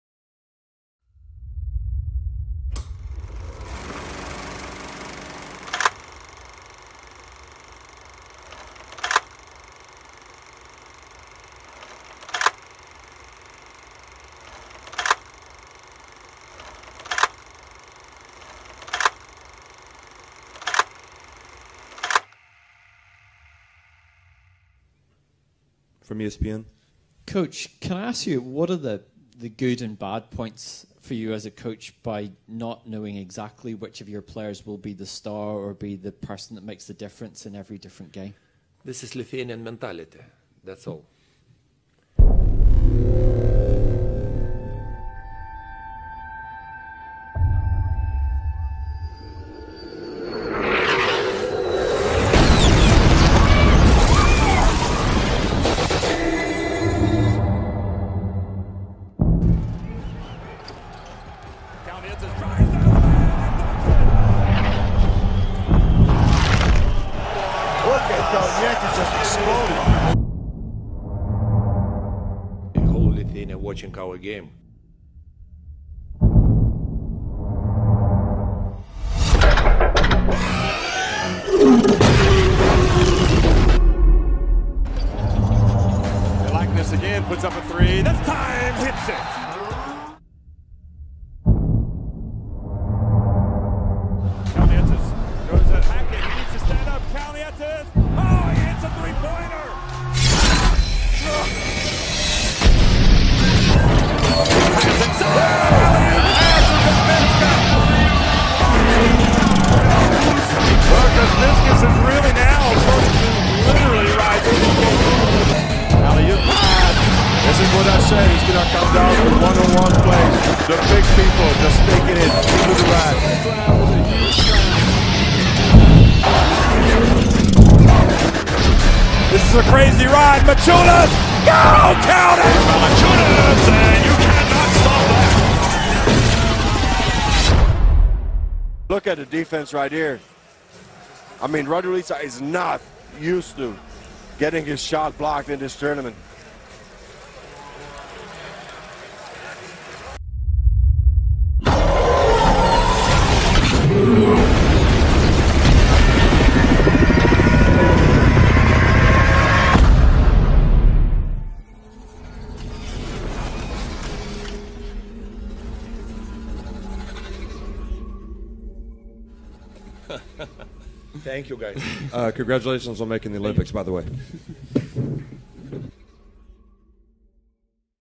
Lithuanian National Basketball Team: Underrated Power (dedicated to ESPN )
3 Olympic bronze in a row, back-to-back EuroCup Finals, 2 consecutive World Cup TOP 4 performances: ESPN (USA) giving Lithuania a ridiculous 8th spot before Eurobasket2015 starts, just because not knowing anybody from their team except Jonas Valanciunas.

This is a personal video message to them.